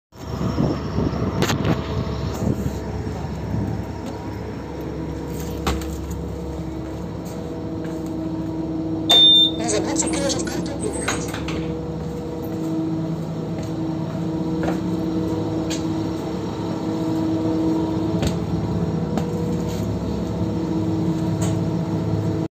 Автобус 28 (Королёв/Мытищи) / Силикатная улица — Станция Мытищи / Салон ЛИАЗ / 21.04.2024
Автобус: ЛИАЗ 529260, 2015 г., госномер ка908 50
Снято на начальной остановке: "Улица Силикатная" в городе Королёв
Также на 28 маршруте я заметил автобус ЛИАЗ 429260, 2023 г. с гос.номером е724ре790. Но в этот день я его поймал только по пути и снять не успел.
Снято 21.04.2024